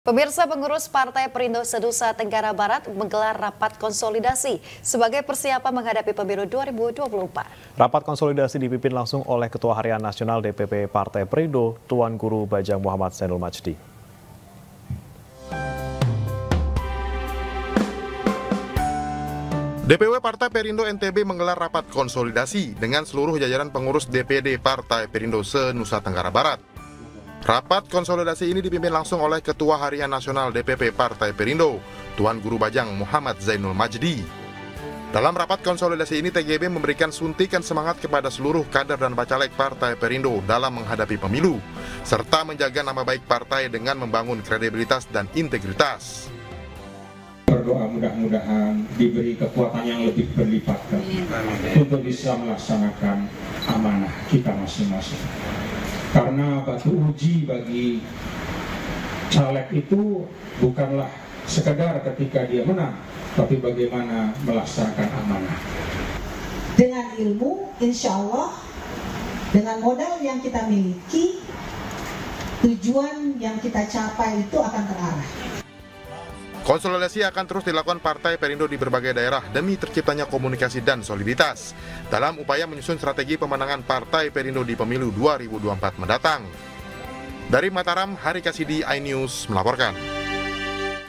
Partai Perindo Rapat Konsolidasi Se-NTB Dipimpin TGB Muhammad Zainul Majdi
Ketua Harian Nasional DPP Partai Perindo TGB Zainul Majdi memberikan suntikan semangat kepada bakal calon legislatif (bacaleg) Partai Perindo di Nusa Tenggara Barat (NTB). Hal tersebut disampaikan TGB dalam rapat konsolidasi bacaleg Partai Perindo di NTB, Kamis (25/5/2023).